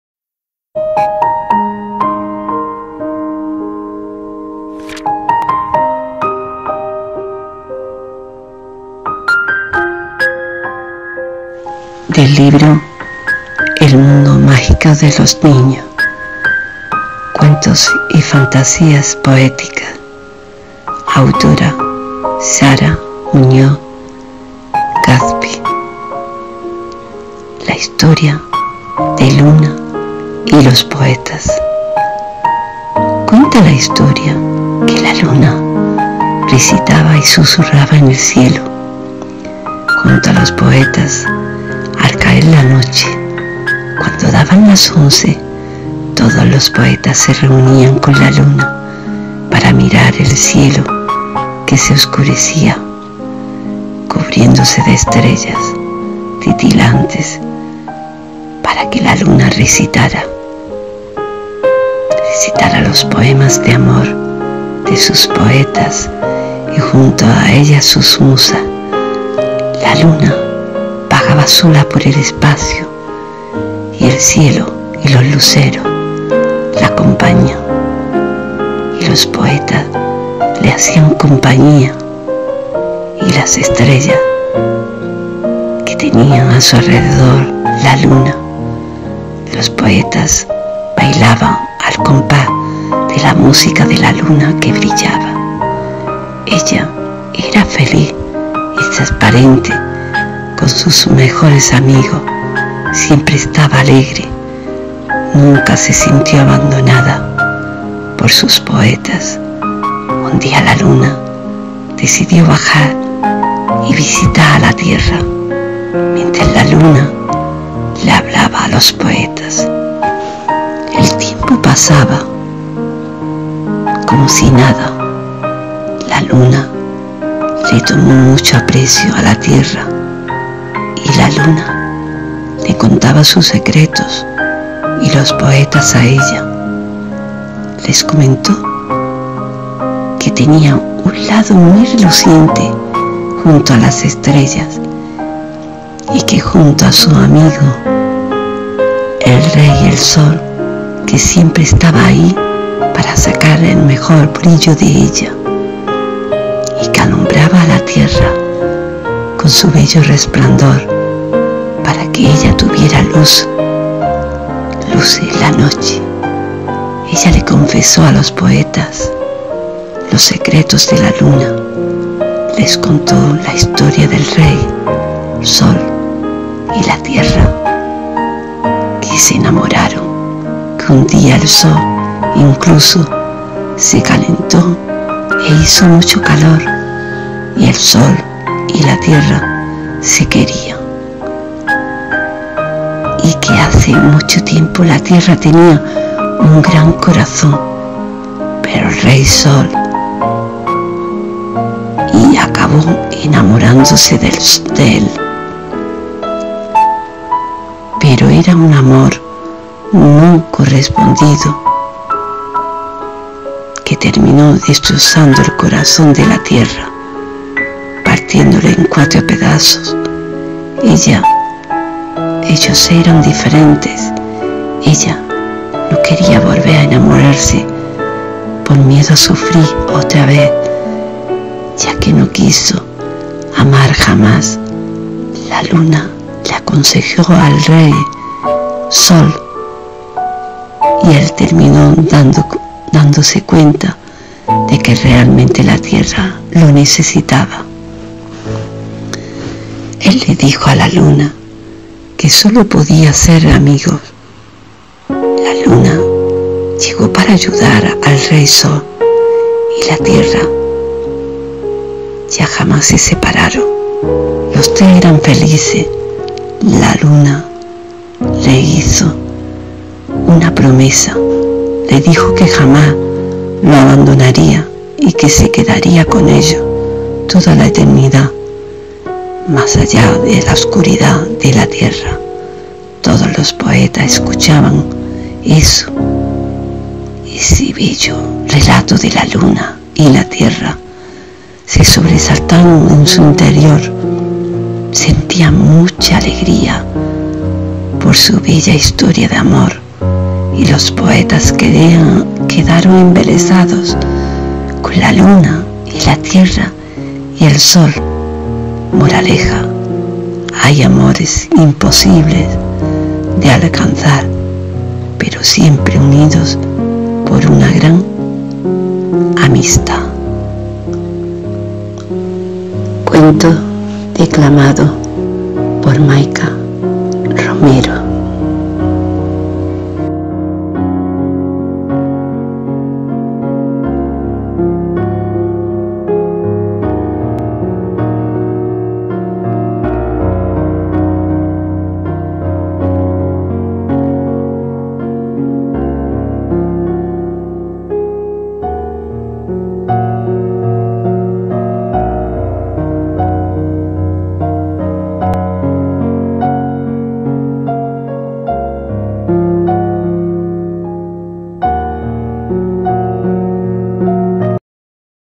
CUENTO DE NIÑOS SARA MUÑOZ GASPIZ DECLAMADO MAIKA ROMERO
Aqui os dejo este nuevo poema en video de esta gran poeta, como es SARA MUÑOZ GASPIZ A DUO CON MAIKAA ROMERO...unaa maravilla siempre sus poemas y sus libros.